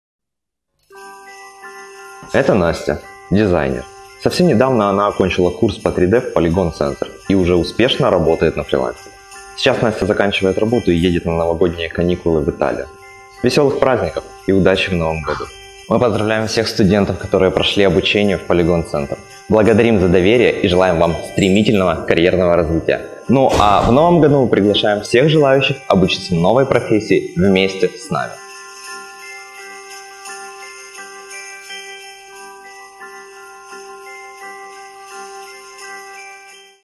З Новим Роком, студенти Polygon Center!
Ми вітаємо всіх студентів Polygon Center з Новим Роком! Бажаємо вам удачі у справі, яку ви розпочали разом з нами!